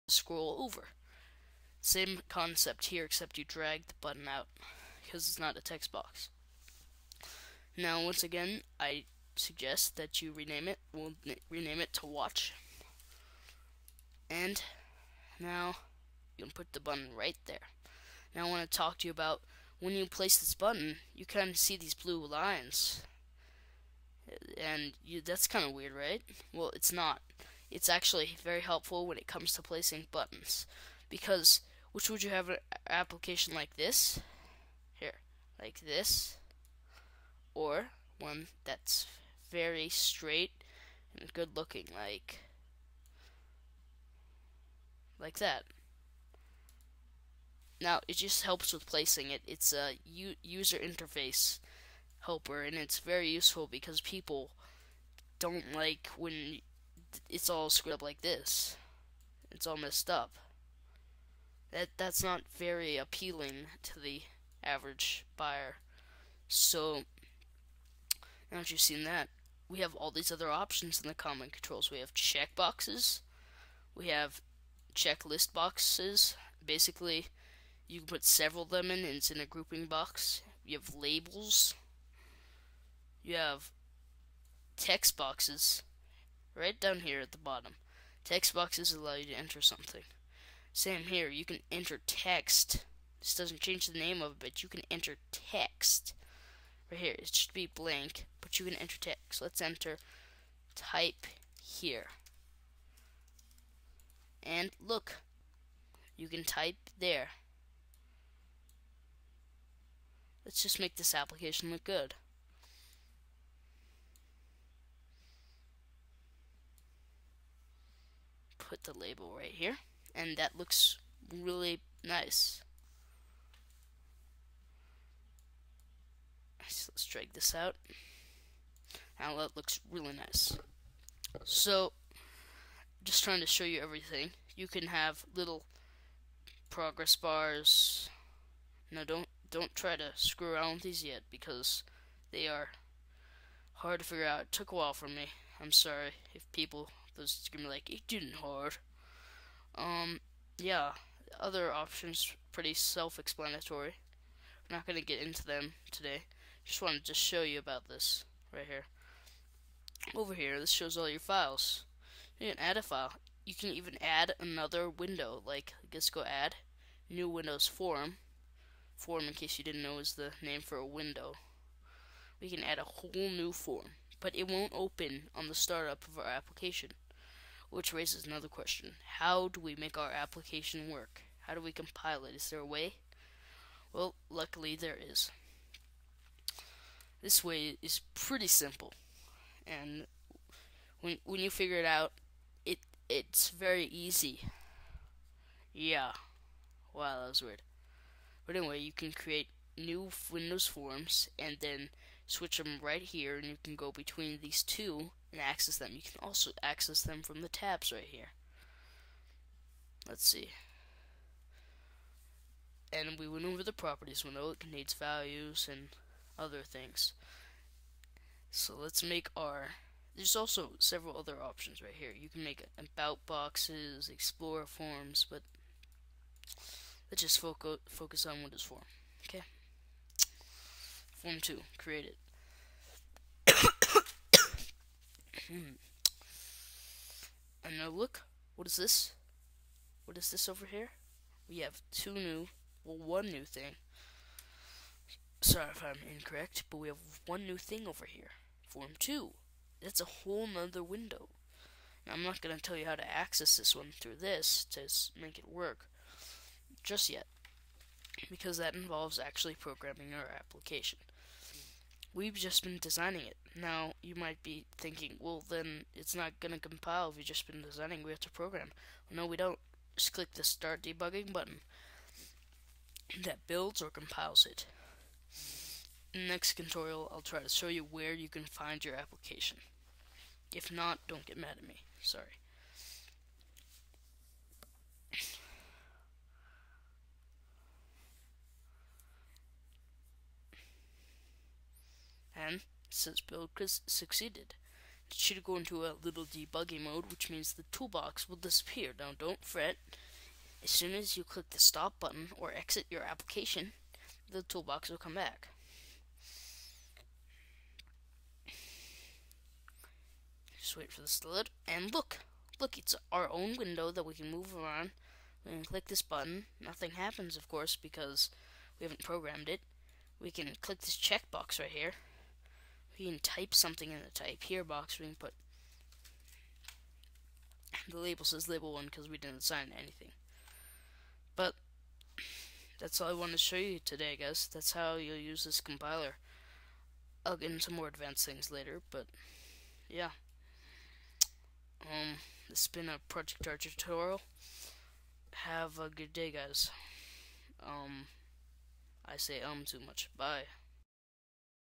Project Argio VB 2008 #3.1: A look at the IDE (Turn volume up)
This is a short over view of what we will be learning in the videos please enjoy!

If you do I will ask you for your email and If you give it to me I will send you weekly code updates :).